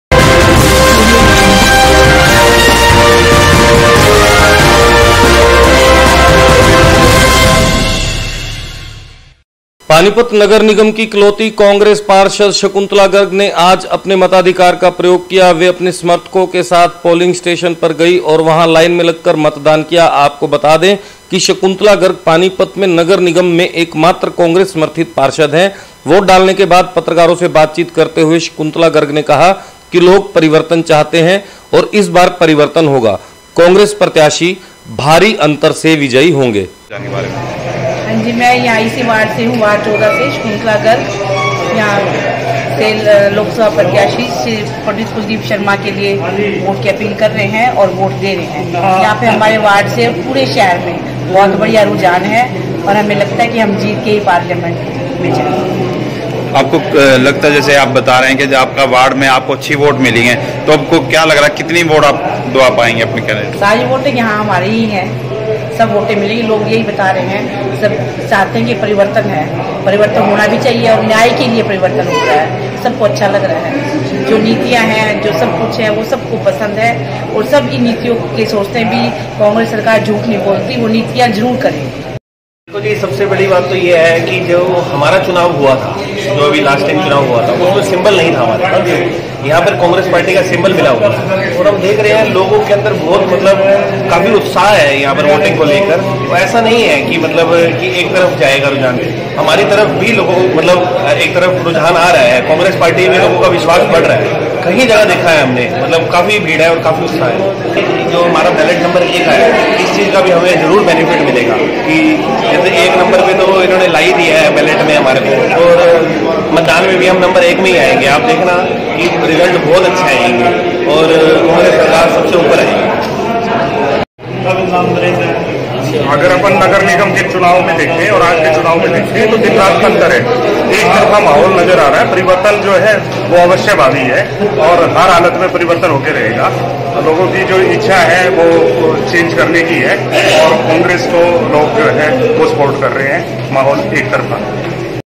वोट डालने के बाद पानीपत नगर निगम की इकलौती कांग्रेस पार्षद ने कहा लोग चाहते हैं परिवर्तन, हम जीतेंगे
PANIPAT AAJKAL :वोट डालने के बाद पानीपत नगर निगम की इकलौती कांग्रेस पार्षद ने कहा लोग चाहते हैं परिवर्तन, हम जीतेंगे